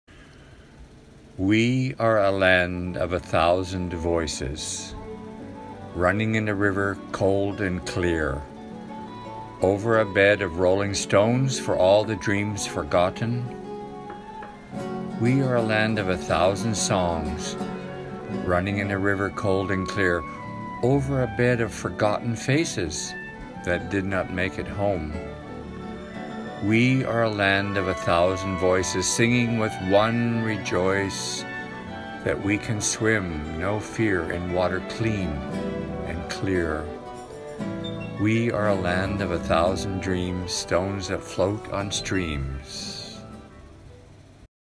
we are a land of 1000 voices
a sonnet to Canada, inspired by the music of Eduardo Angulo  and the creations of Serge Bennathan. Acrylic on canvas. 30"x30".  76.2cm x 76.2 cm, unframed.

Brian Peterman, September 2020